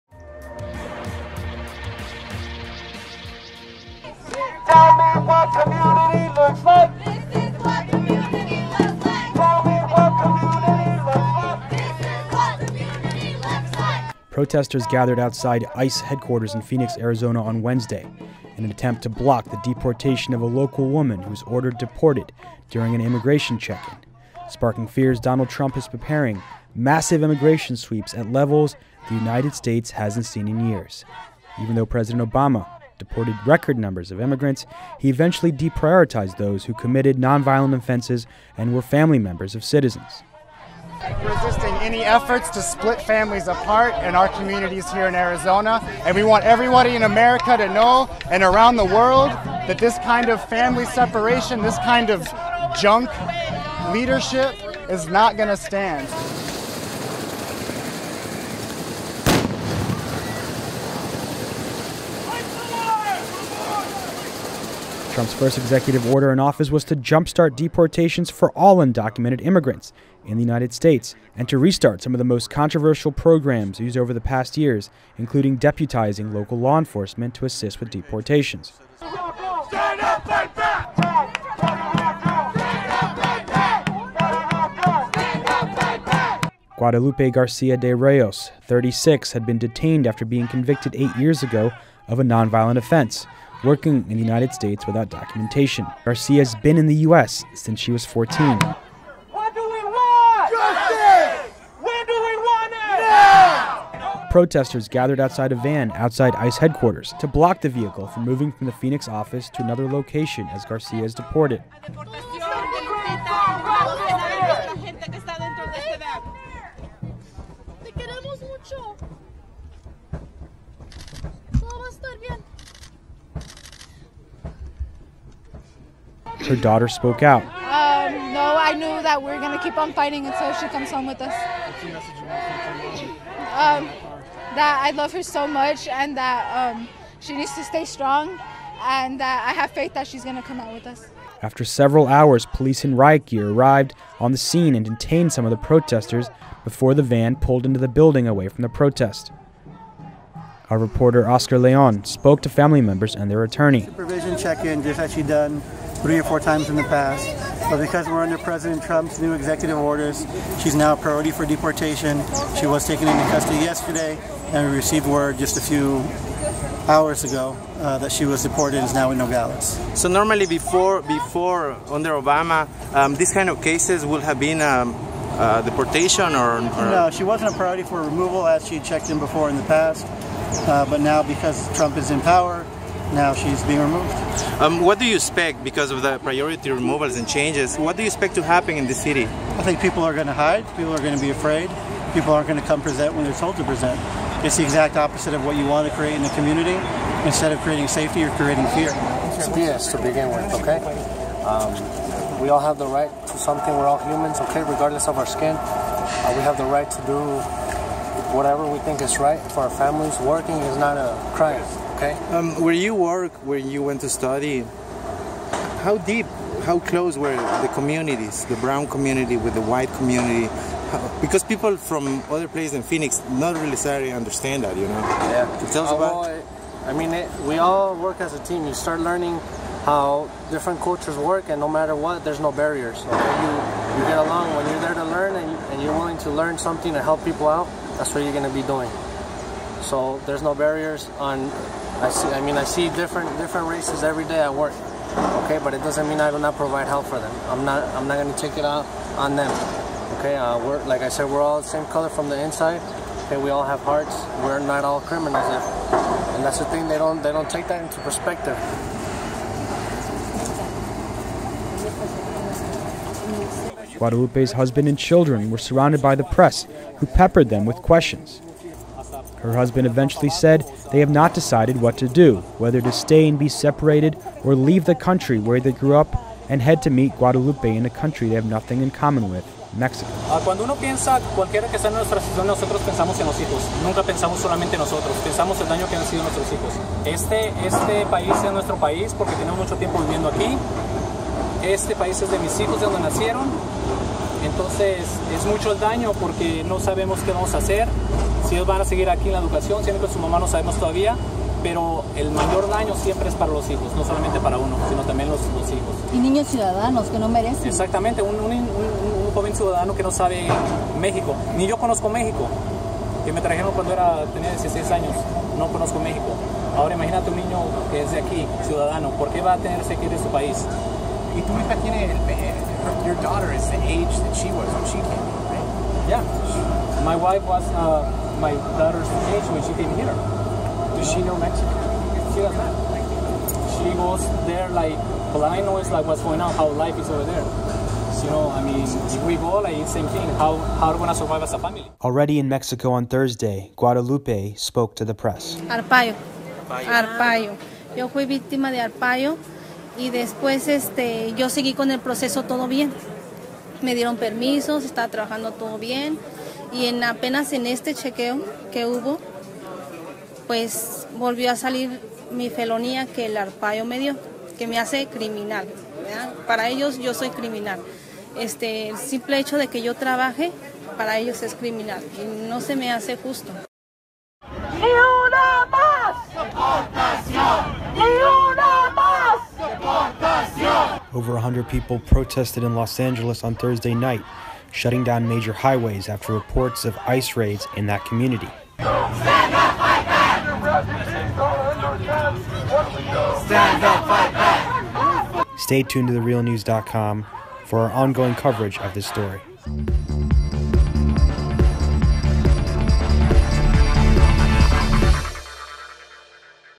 Deportation of Phoenix Woman Sparks Fears of Massive Immigration Sweeps
TRNN Reporter Oscar Leon speaks to the family of Guadalupe Garcia de Rayos and their attorney